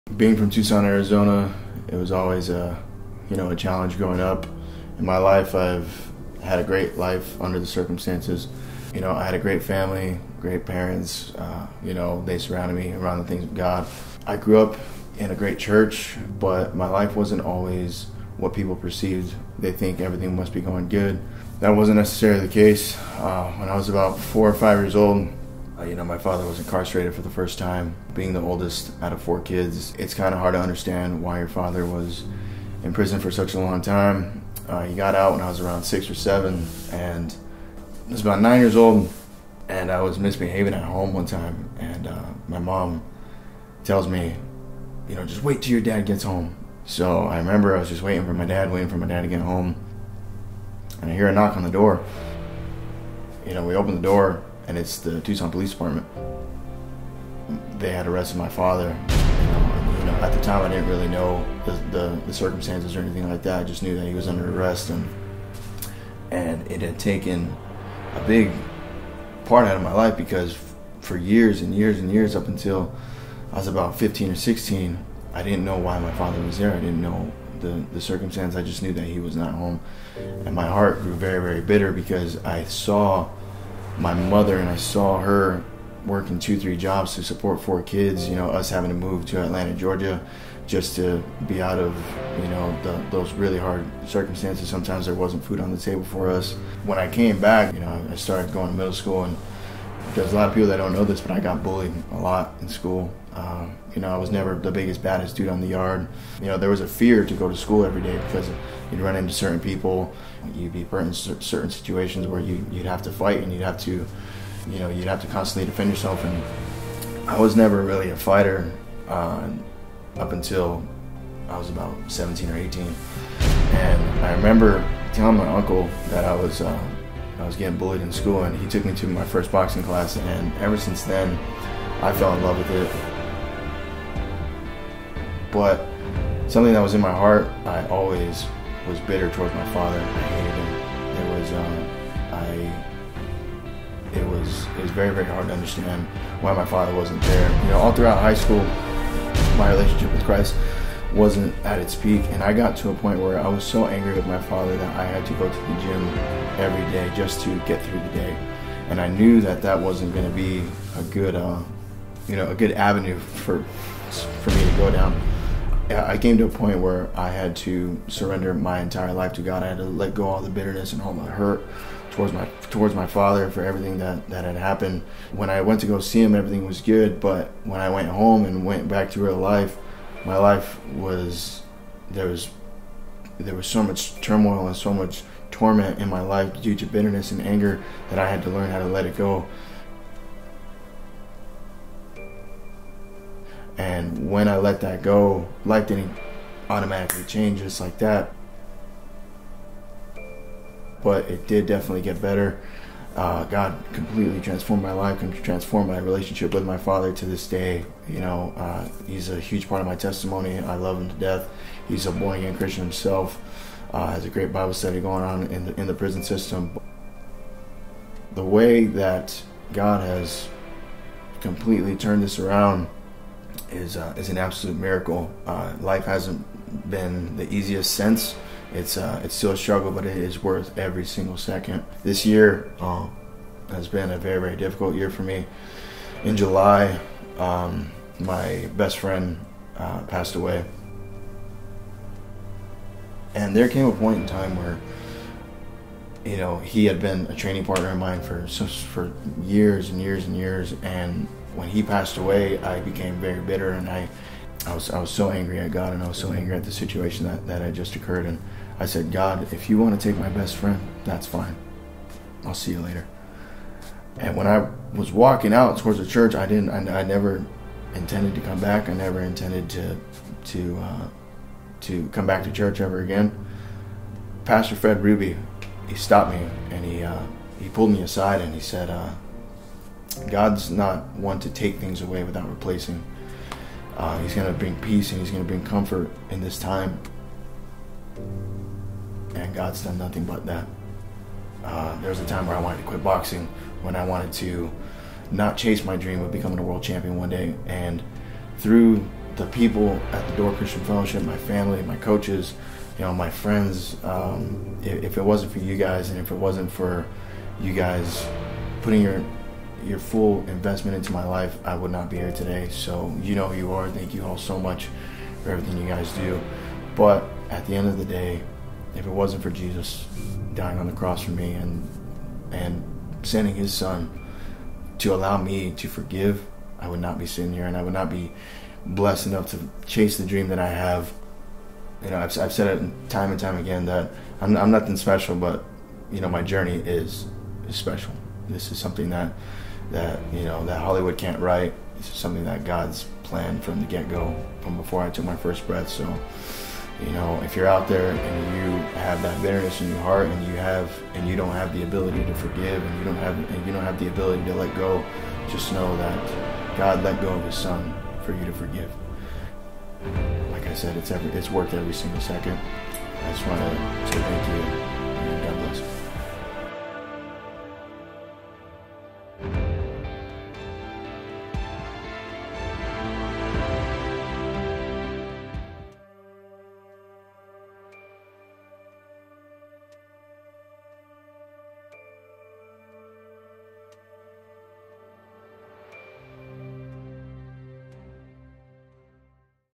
BITTERNESS WAS DESTROYING MY LIFE - TESTIMONY
Episode 3 Daniel Lopez shares how he was bitter at his father for years, and bitter at God for losing his best friend. God faithfulness has restored his heart and broken that bitterness.

The Lens I Use

How I get Audio